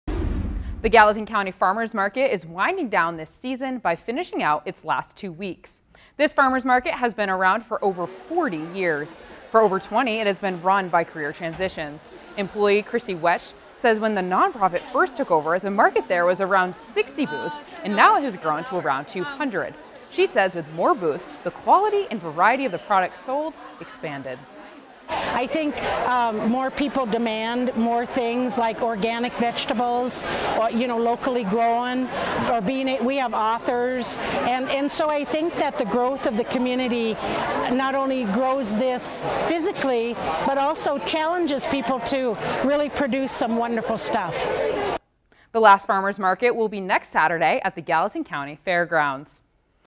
Gallatin Valley Farmers' Market winds down 2018 season
The Gallatin Valley Farmers’ Market is winding down the last two weeks of its 2018 season.